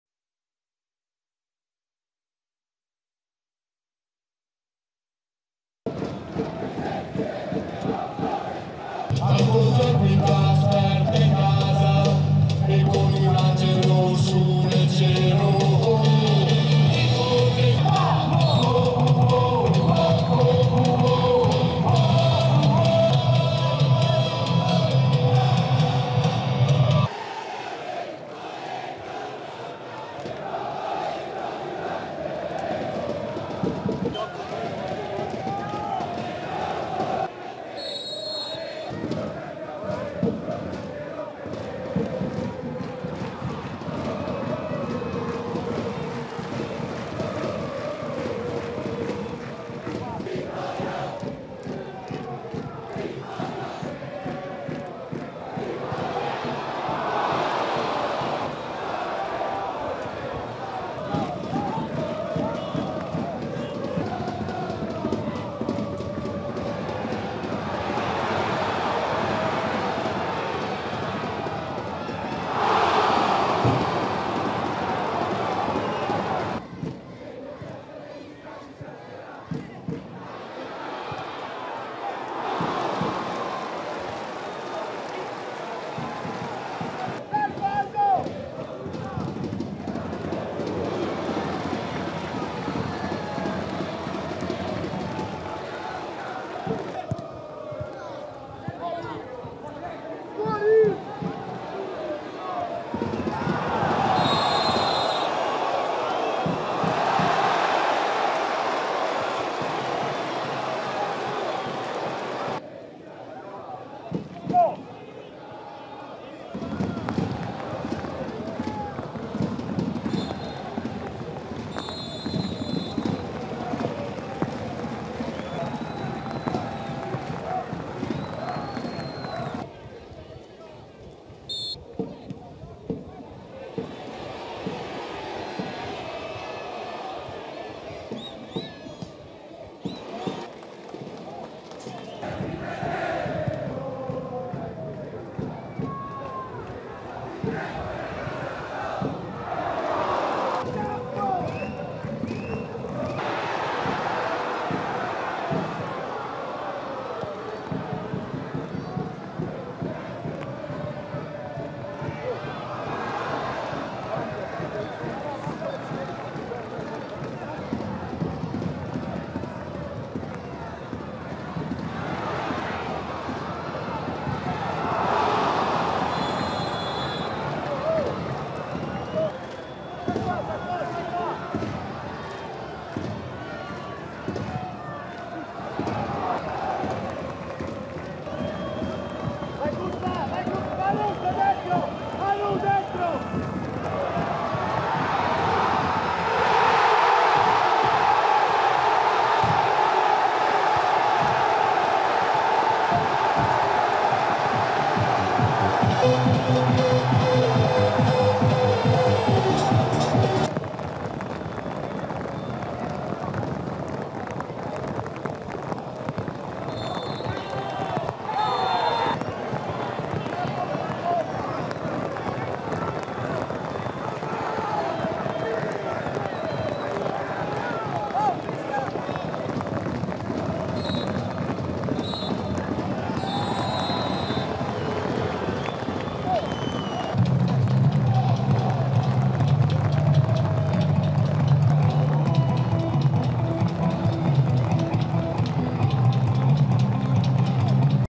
Cittadella-Cosenza 0-0 | Serie BKT | Il Cosenza resiste in inferiorità numerica
Nonostante l'uomo in meno dal 37' per via dell'espulsione di Venturi, il Cosenza resiste e porta a casa un pareggio che muove di poco la classifica.

Il Cittadella chiude la prima partita con Dal Canto in panchina con il rimpianto del goal annullato a Pavan negli ultimi minuti.

Ecco gli highlight del match tra Cittadella e Cosenza.